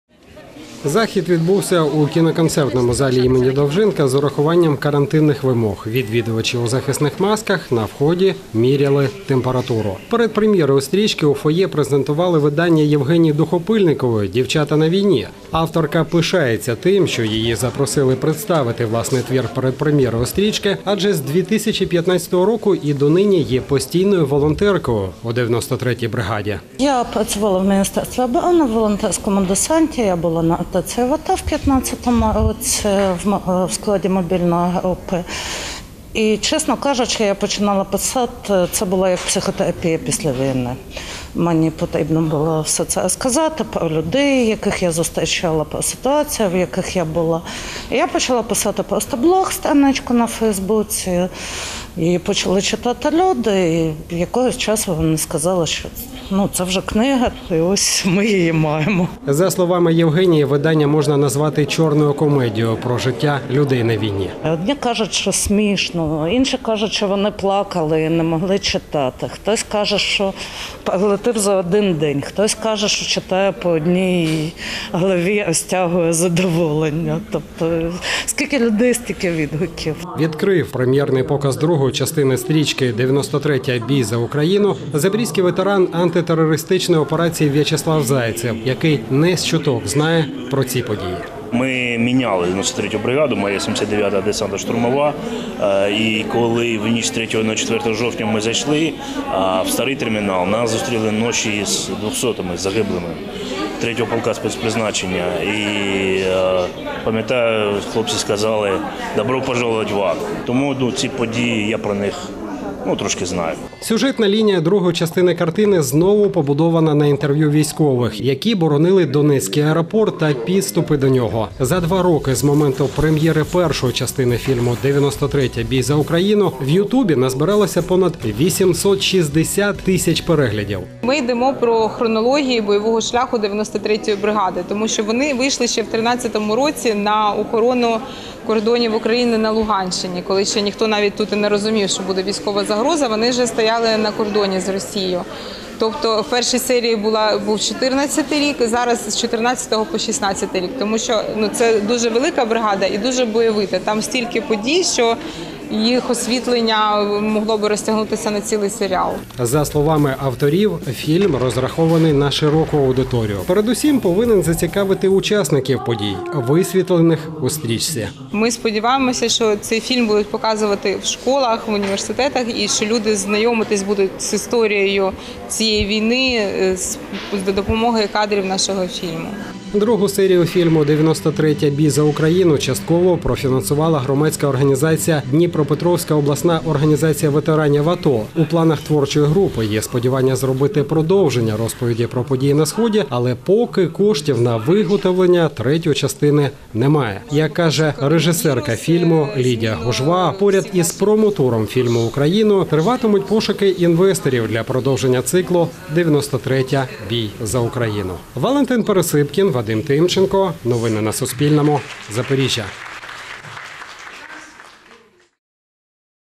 Новини - Фільм та книга - 13.07.2020
У Запоріжжі відбулися прем’єра другої частини документального фільму «93: бій за Україну» та презентація книги «Дівчата на війні». Об’єднаний захід організатори презентували 12 липня, у кіноконцертному залі імені Олександра Довженка.